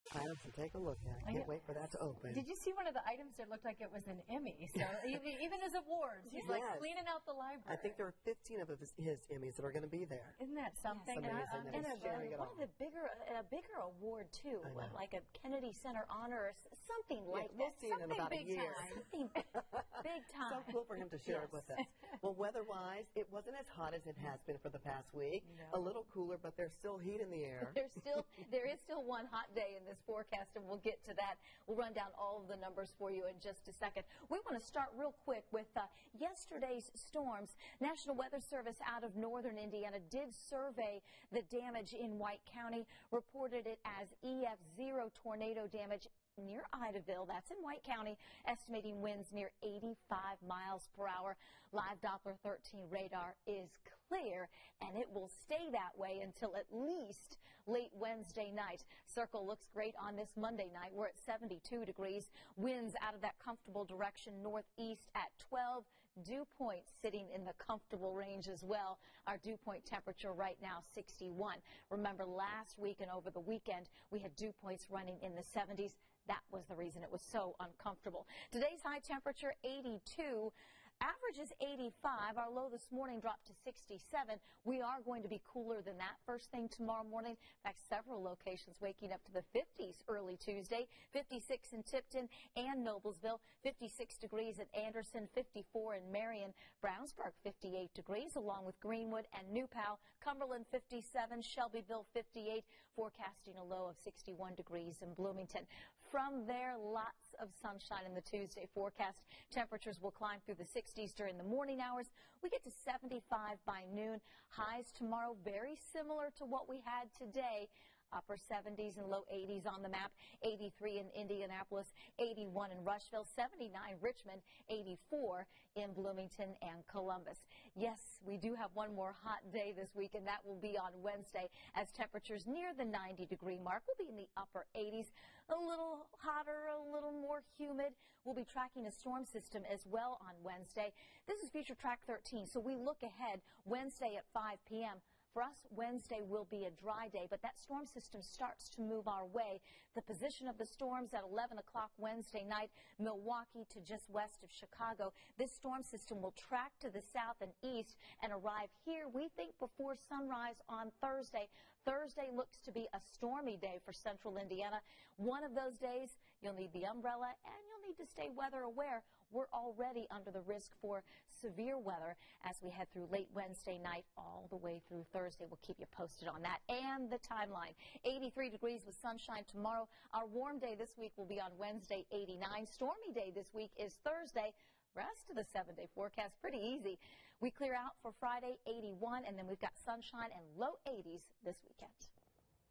11pm Forecast
Angela says we're in for some cooler, dry days ahead.